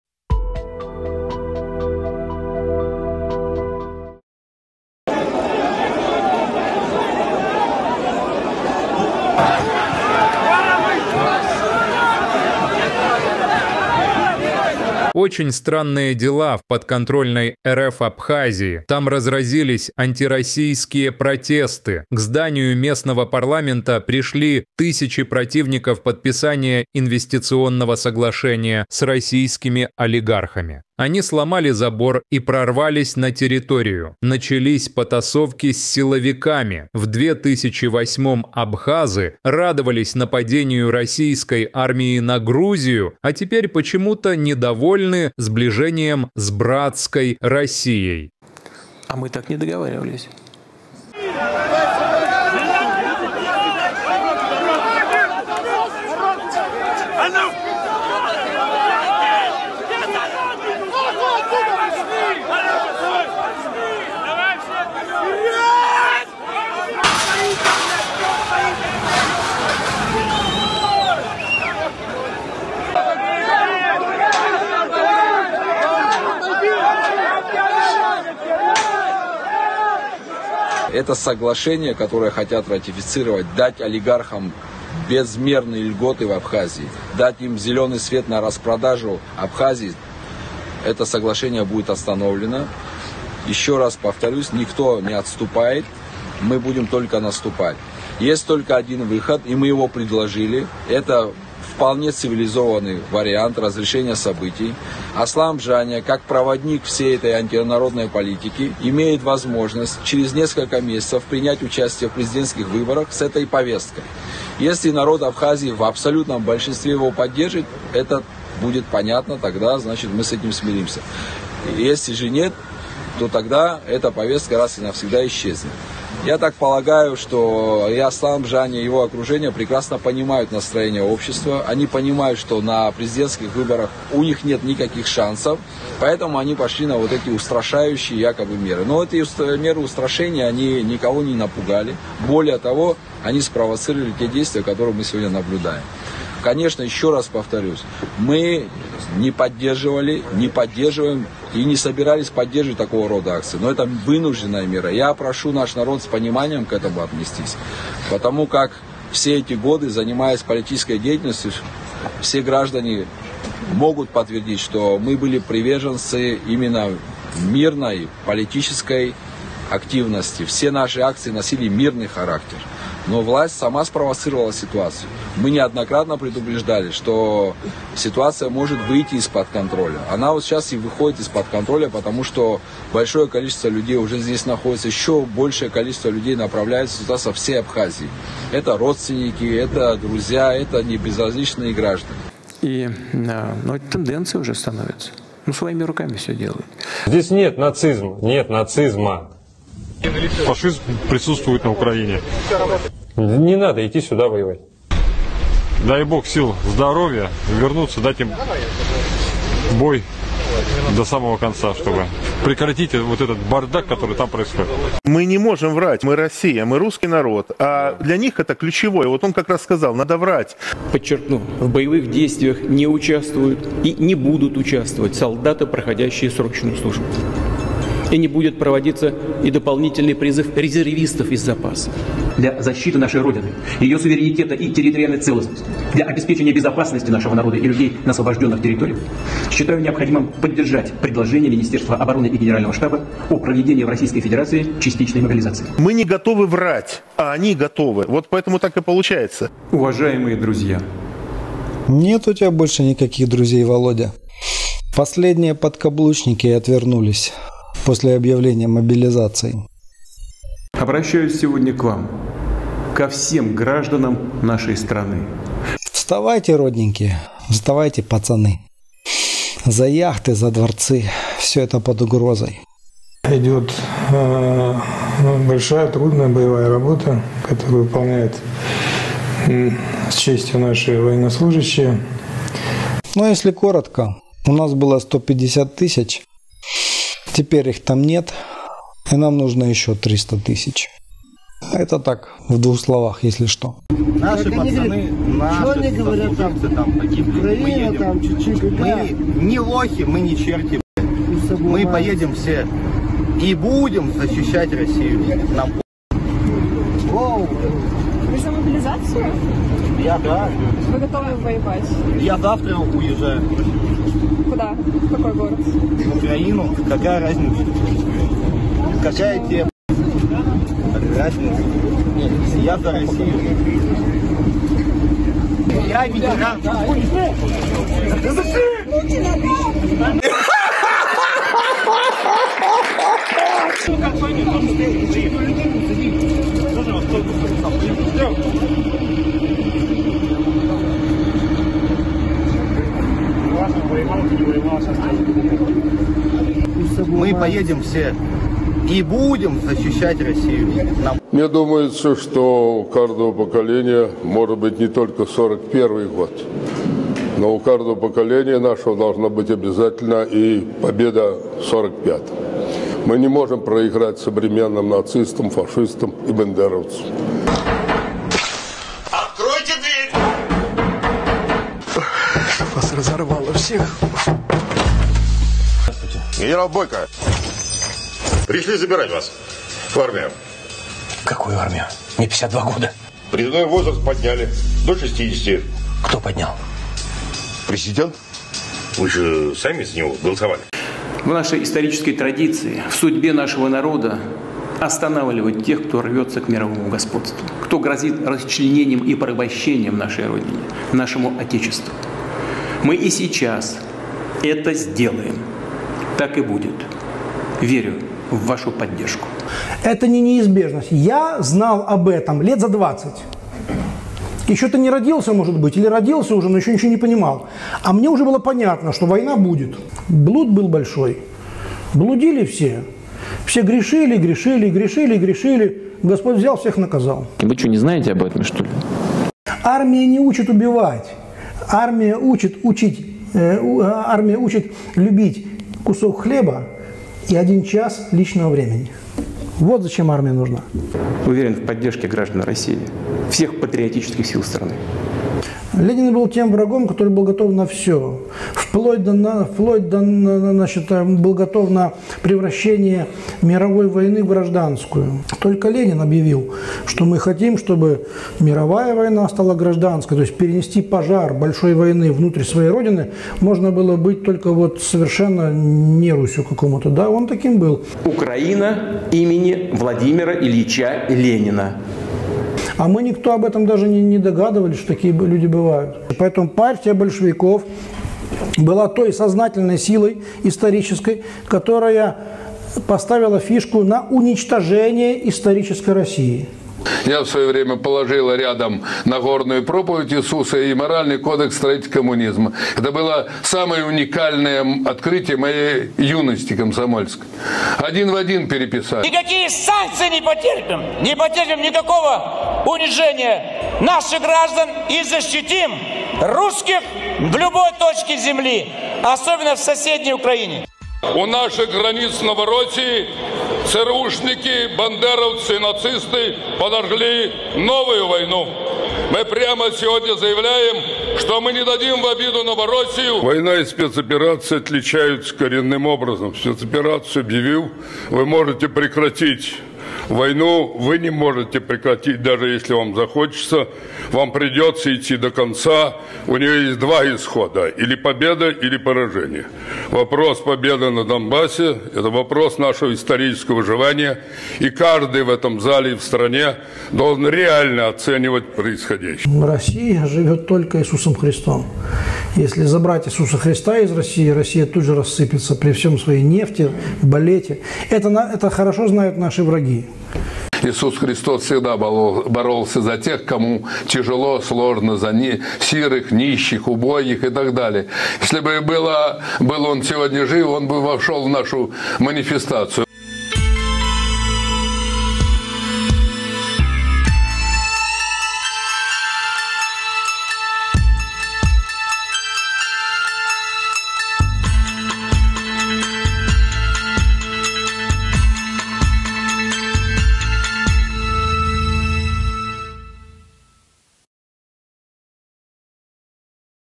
Майдан в Абхазии. Выгоняют российских олигархов
Протестующие жители Абхазии снесли забор парламента и бросают в полицию бутылки. Они требуют отклонить проект о ратификации инвестиционного соглашения с Россией. В рамках этого документа российским компаниям предоставляется право реализовывать проекты в Абхазии и получать в собственность землю и недвижимость.

 Президент Бжания сбежал.

Поддержать Om TV:
USDT (TRC) - TYyCeQ7eBadoLMKhZZ6951T83f9V9WoXBC
BTC - bc1q9h2r267ry99pfs38cyceyglu3ayrsalyr9u48z